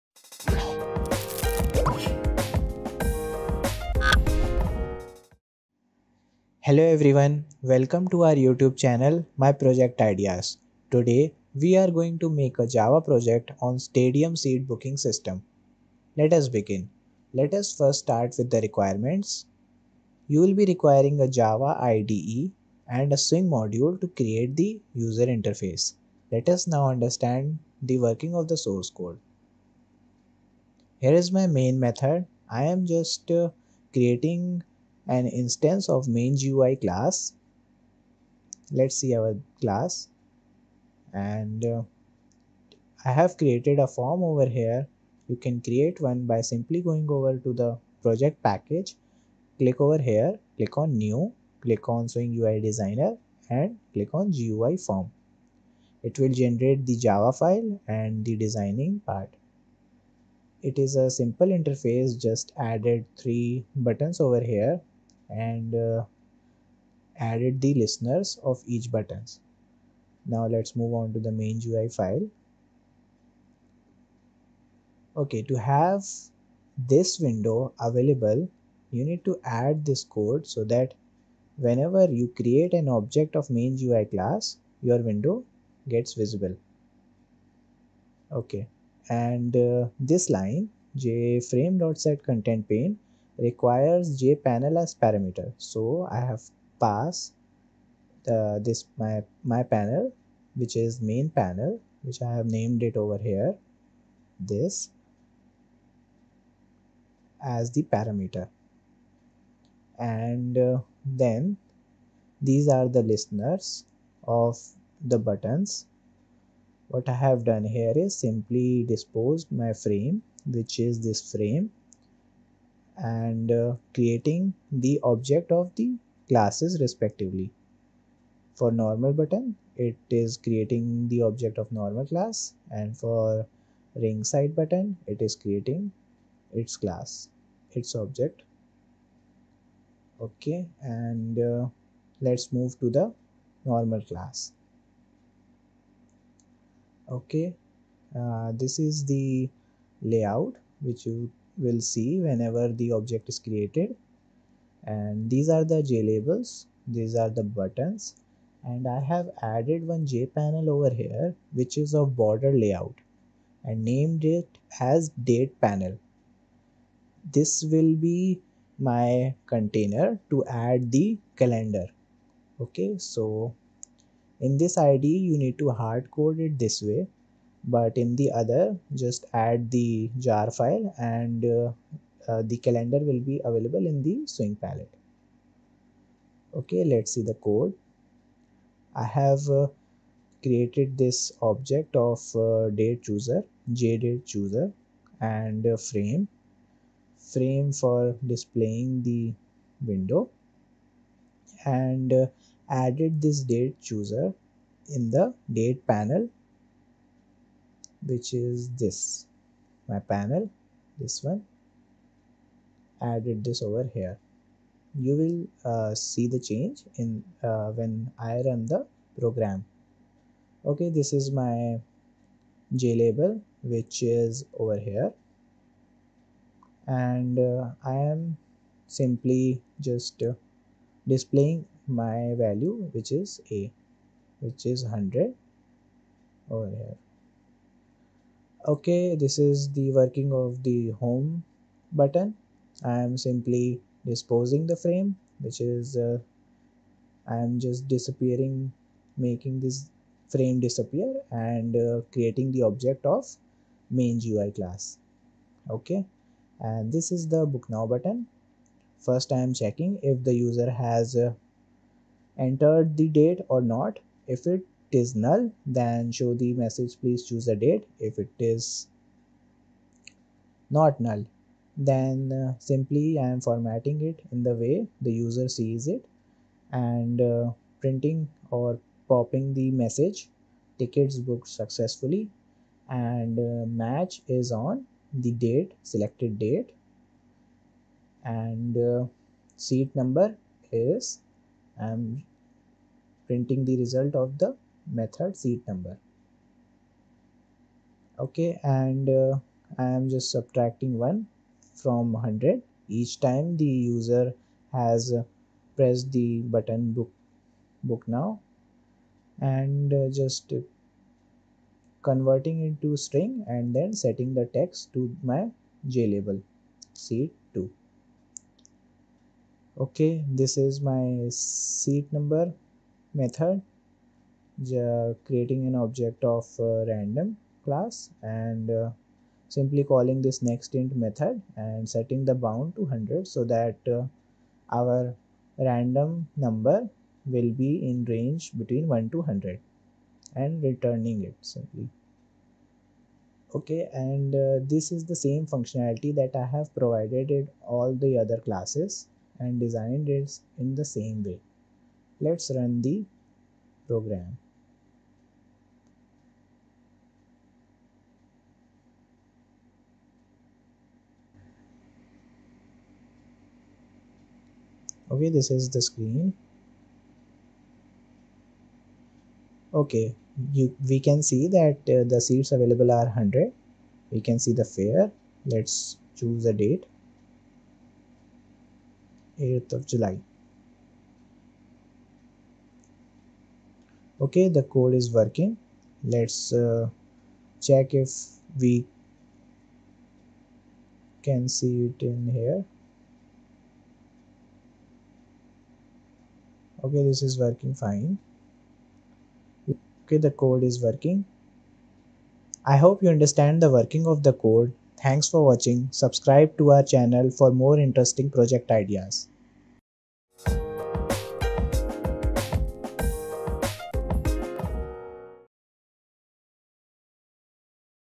Stadium Seat Booking System Using Swing | Java Project
In this coding project, we will create a Stadium Seat Booking System In Java that will book a seat for the user on the selected date. To implement this java project, we will apply a logic to assign a random seat number & keep track of seats booked. SQL database will be implemented to store information related to the booking, and the Swing module will enhance the user interface.

Objectives:

1. We have to apply a seat booking logic that provides a seat number & keeps track of seats available.
2. Make the user interface more interactive with the Swing module.

Requirements:

1. Java Compiler IDE
2. Swing Module

Firstly GUI:

1. The main page consists of 3 buttons named Normal, End stands & Ringside, which will redirect to respective screens.

2. Each screen consists of 3 JLablels with seats available, Fare & Date of Game information.

3. It also has a Jcalendar to select the date of the game.

Moving to the seat booking, we will apply the following rules:

1. Generate a random number between the range 1 to 100; that will be our seat number.

2. Showing popup with confirmation of seat booking.

3. Finally, updating the seats available on the home screen.

Timestamp:

00:13 - Introduction
00:31 - Code Explanation
06:03 - Demonstration